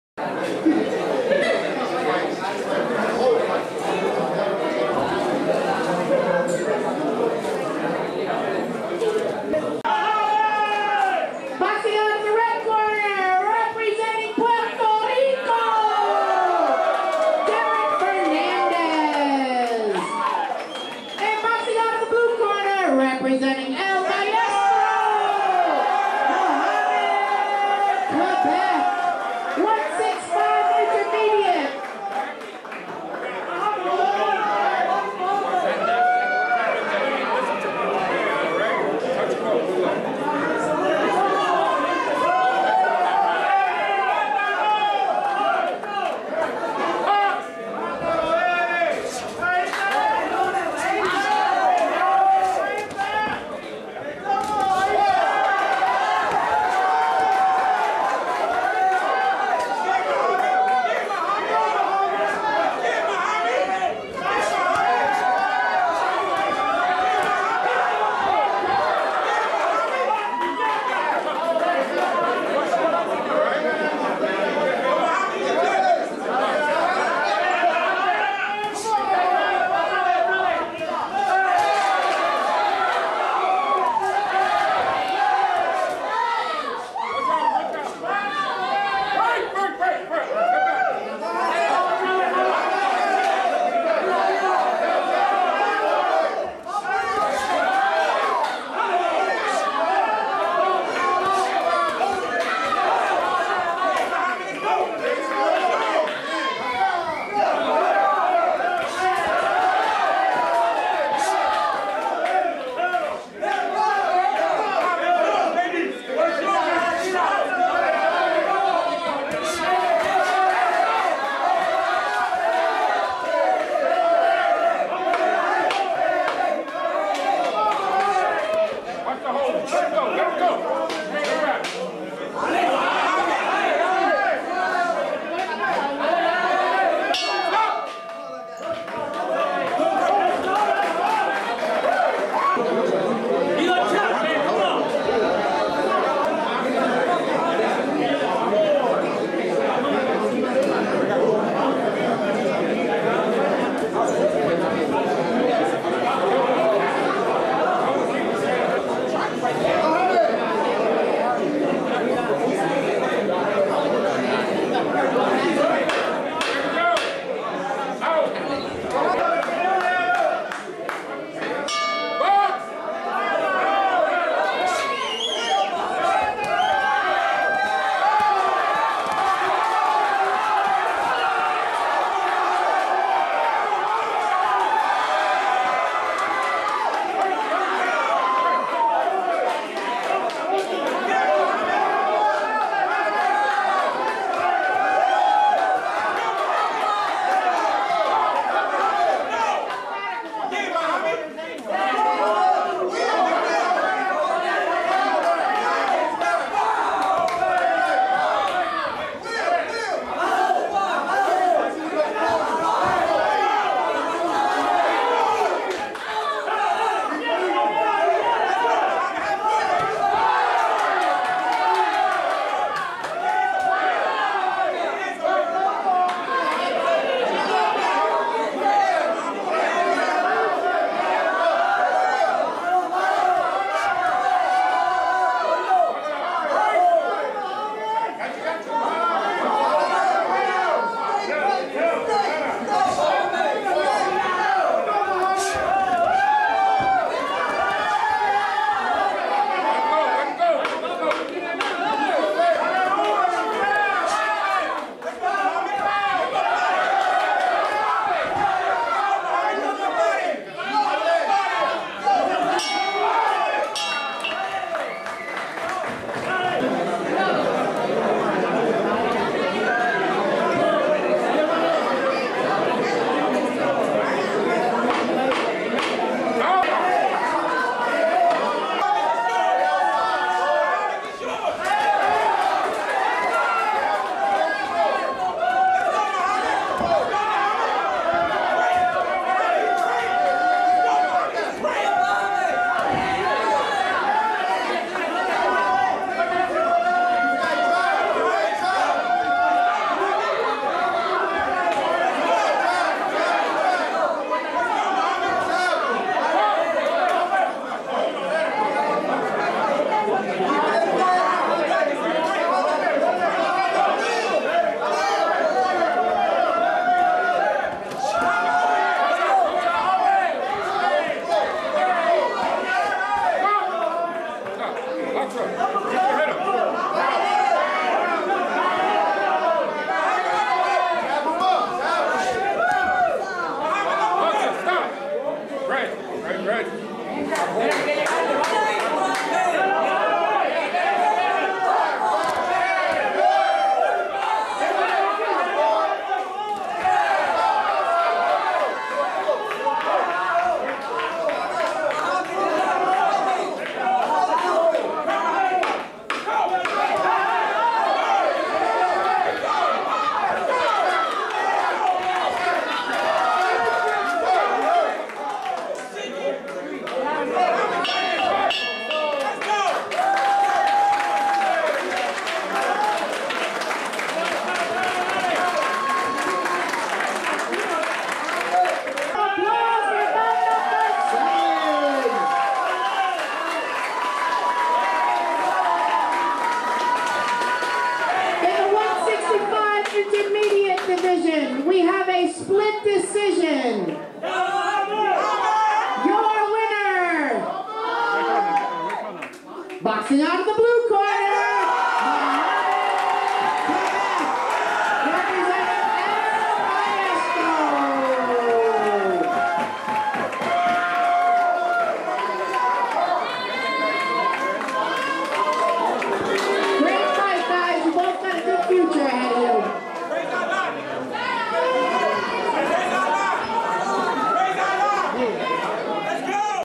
Derek Fernandez : El Maestro Boxing : Mohamad Kebbeth. 3 rounds
USA Boxing @ El Maestro 12/4/21 : Deric Fernandez (R) / Mohamad Kebbeth (B) : 165 lb. 3 rounds